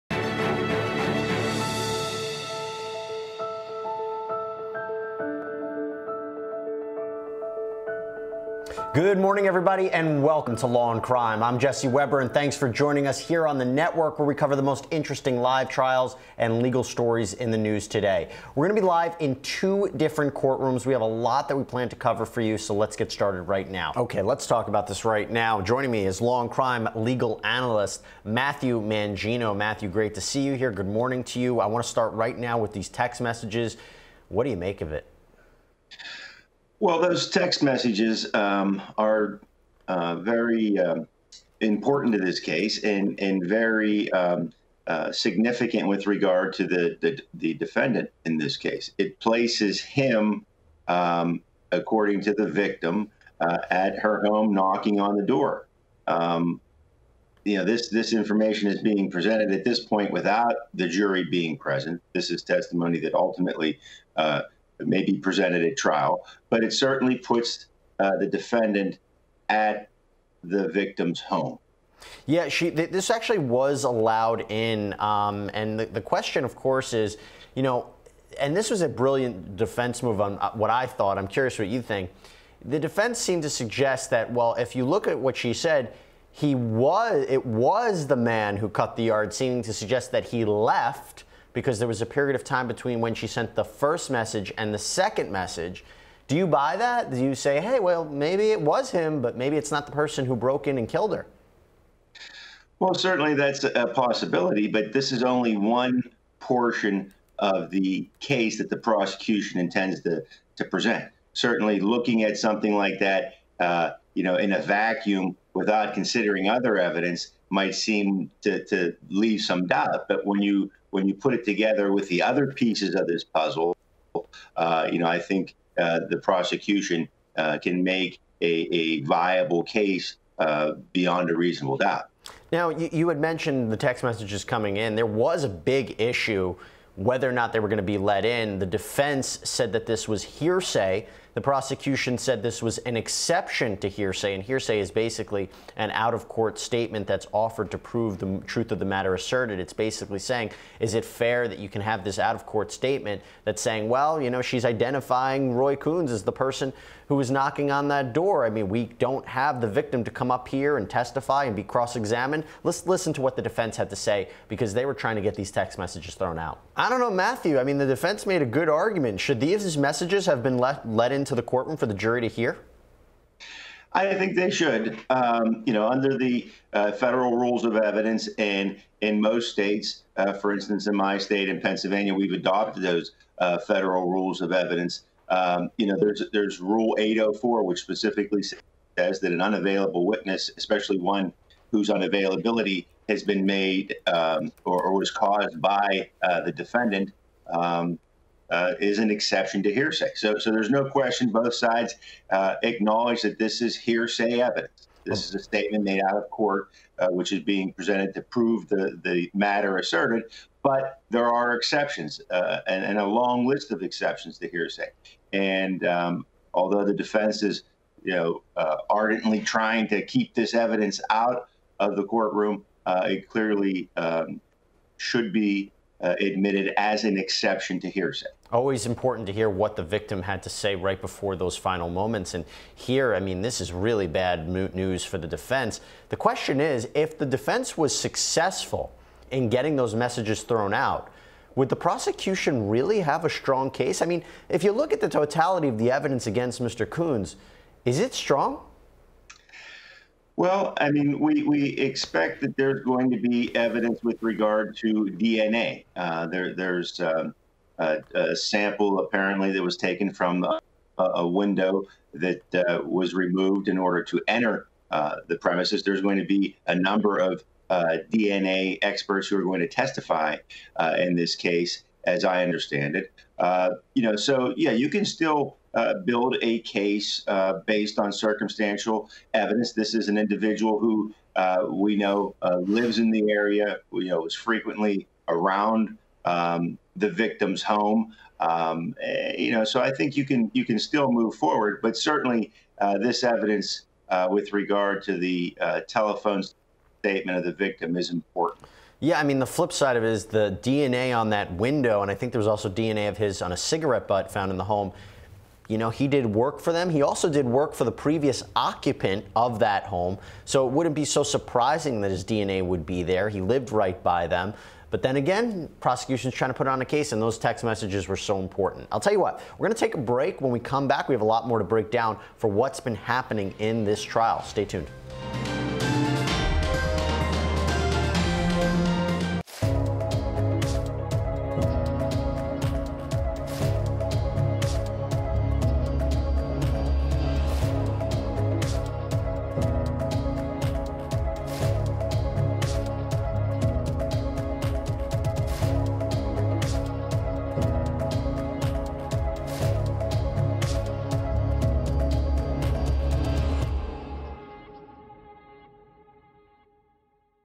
Jesse Weber & Legal Analyst Matthew Mangino Discuss the Text Messages in the Roy Coons Trial 7/10
Jesse Weber & Legal Analyst Matthew Mangino Discuss the Text Messages in the Roy Coons Trial